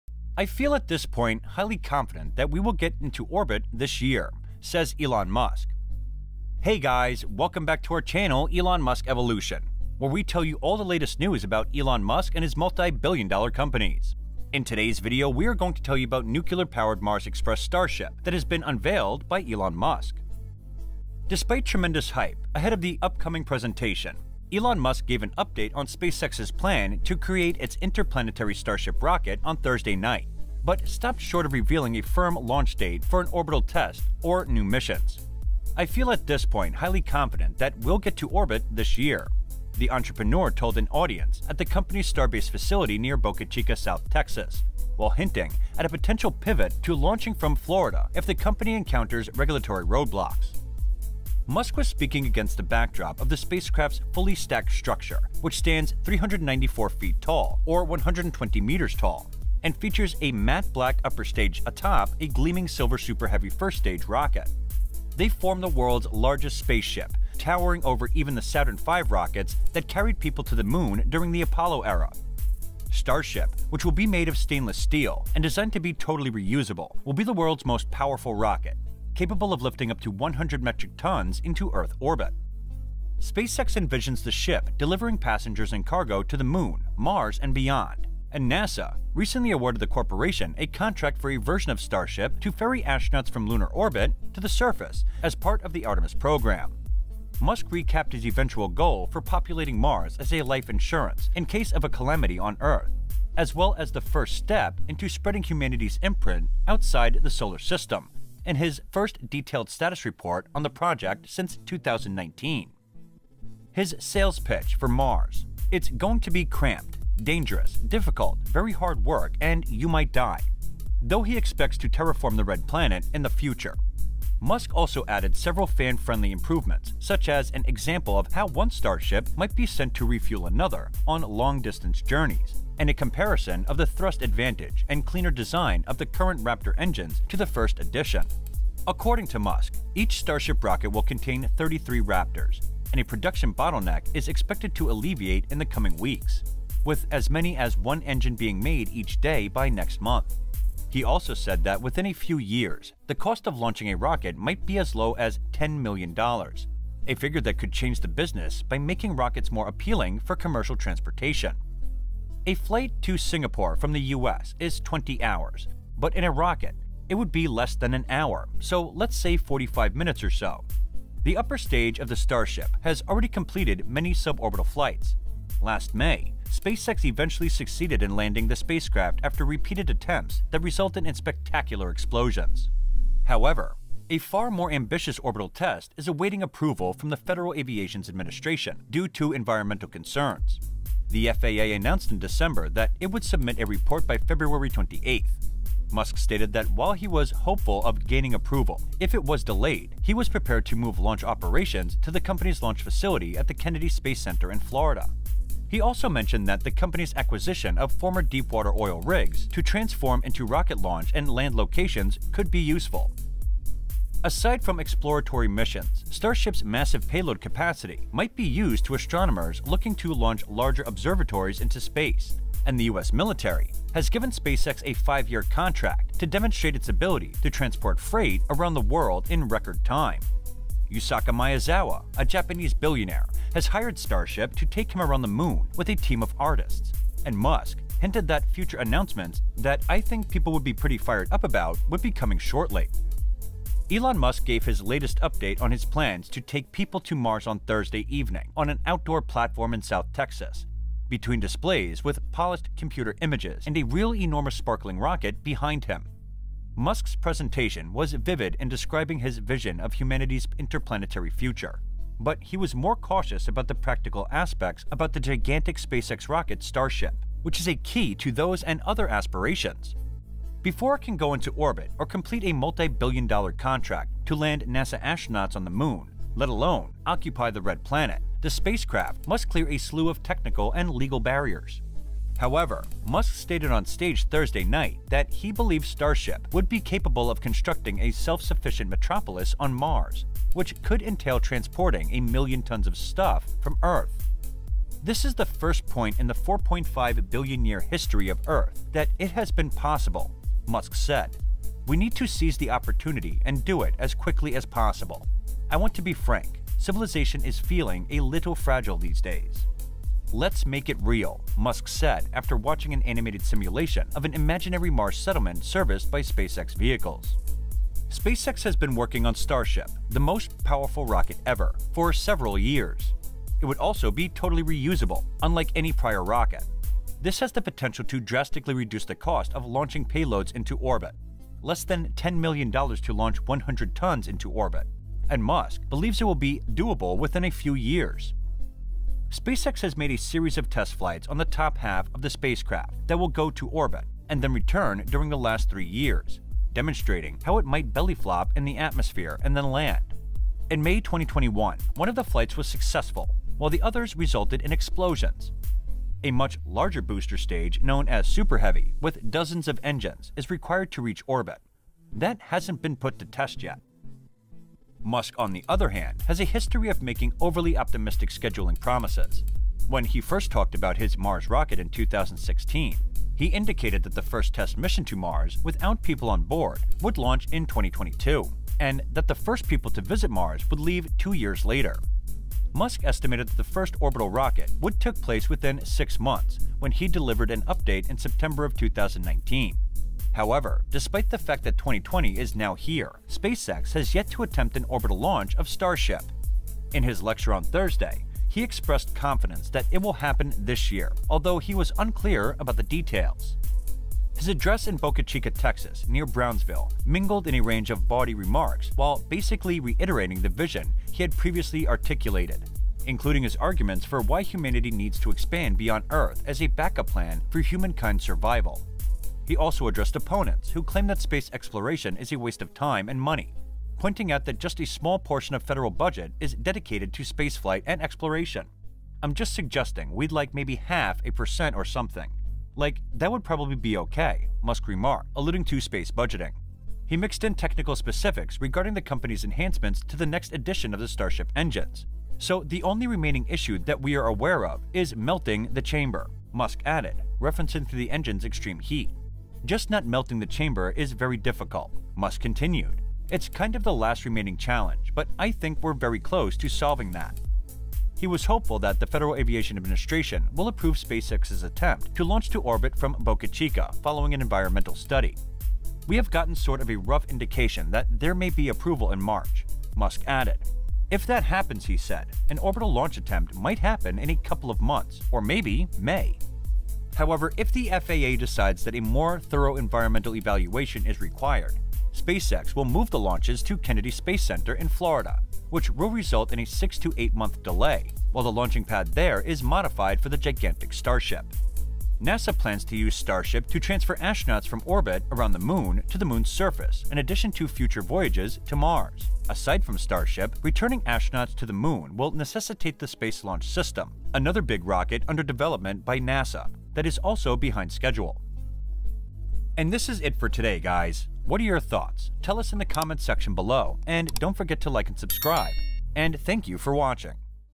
Elon Musk Just DROPPED A BOMBSHELL To Jeff Bezos With This Mars Express!
“I feel at this point highly confident that we´ll get to orbit this year,” says Elon Musk.  Hey guys! In today’s video we are going to tell you about nuclear powered Mars express starship that has been unveiled by Elon Musk.

Despite tremendous hype ahead of the uncommon presentation, Elon Musk gave an update on SpaceX's plans to create its interplanetary Starship rocket on Thursday night, but stopped short of revealing a firm launch date for an orbital test or new missions. “I feel at this point highly confident that we´ll get to orbit this year,” the entrepreneur told an audience at the company's Starbase facility near Boca Chica, South Texas, while hinting at a potential pivot to launching from Florida if the company encounters regulatory roadblocks.

Musk was speaking against the backdrop of the spacecraft's fully-stacked structure, which stands 394 feet (120 metres) tall and features a matte black upper-stage atop a gleaming silver Super Heavy first-stage rocket. They form the world's largest spaceship, towering over even the Saturn V rockets that carried people to the Moon during the Apollo era. Starship, which will be made of stainless steel and designed to be totally reusable, will be the world's most powerful rocket, capable of lifting up to 100 metric tonnes into Earth orbit.

SpaceX envisions the ship delivering passengers and cargo to the Moon, Mars, and beyond, and a space agency recently awarded the corporation a contract for a version of Starship to ferry astronauts from lunar orbit to the surface as part of the Artemis program. Musk recapped his eventual goal for populating Mars as a “life insurance” in case of calamity on Earth, as well as the first step in spreading humanity's imprint outside the solar system, in his first detailed status report on the project since 2019.

His “sales pitch” for Mars: “It´s going to be cramped, dangerous, difficult, very hard word, you might die” — though he expects to terraform the Red Planet in the future. Musk also added several fan-friendly improvements, such as an example of how one Starship might be sent to refuel another on long-distance journeys, and a comparison of the thrust advantage and cleaner design of the current Raptor engines to the first edition.

According to Musk, each Starship rocket will contain 33 Raptors, and a production bottleneck is expected to alleviate in the coming weeks, with as many as one engine being made each day by next month. He also said that within a few years, the cost of launching a rocket might be as low as $10 million, a figure that could change the business by making rockets more appealing for commercial transportation.

A flight to Singapore from the US is 20 hours “but in a rocket it would be less than an hour. So, let's say 45 minutes or so.” The upper stage of the Starship has already completed many suborbital flights. Last May, SpaceX eventually succeeded in landing the spacecraft after repeated attempts that resulted in spectacular explosions.

However, a far more ambitious orbital test is awaiting approval from the Federal Aviation Administration because to environmental concerns. The FAA announced in December that it would submit a report by February 28. Musk stated that while he was hopeful of gaining approval, if it was delayed, he was prepared to move launch operations to the company's launch facility at the Kennedy Space Center in Florida. He also mentioned that the company's acquisition of former deepwater oil drilling rigs to transform into rocket launch and land locations could be useful.